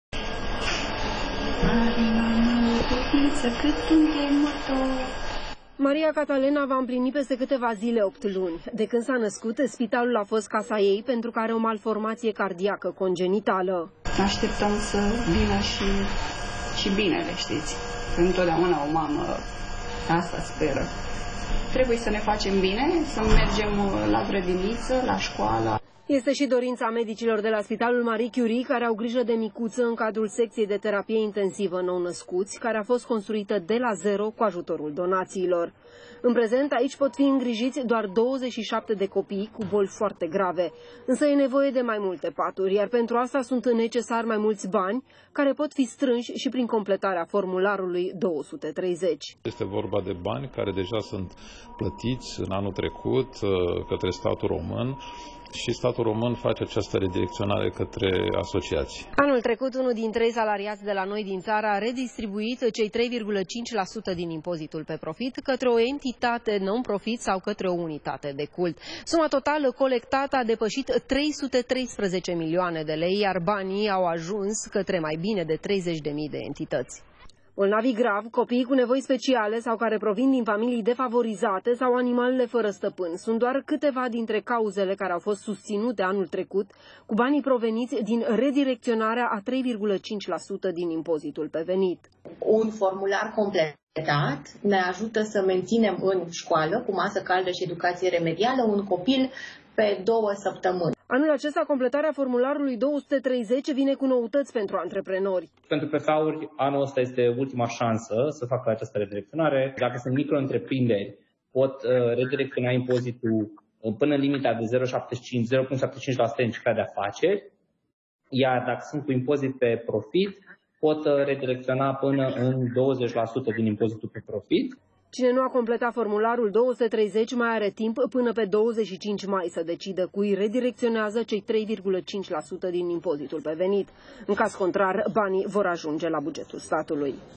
Formularul 230 salvează vieți
Cu un simplu gest putem schimba vieţile celor mai puţin norocoşi dintre noi. Tot ce trebuie să facem este să redirecţionăm o parte din impozitul pe venit pe care îl datorăm statului, completând formularul 230. Cu toate acestea, anul trecut, mai puțin de o treime dintre angajați au făcut acest lucru.